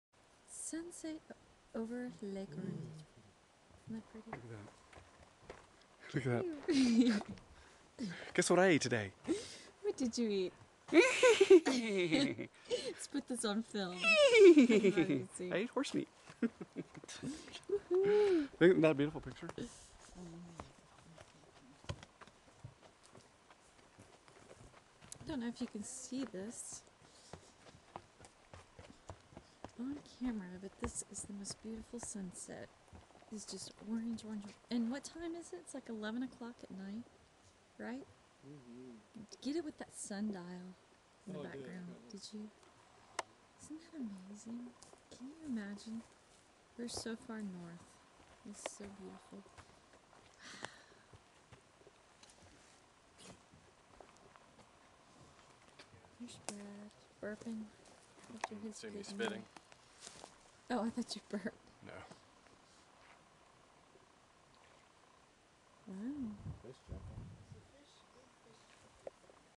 Lake Runn at sunset
Desiree is walking around with the camera.  Ruben discusses our dinner, and Birgitta and Brad make appearances.  Lake Runn is where Birgitta and the Swedish side of the family have summer houses.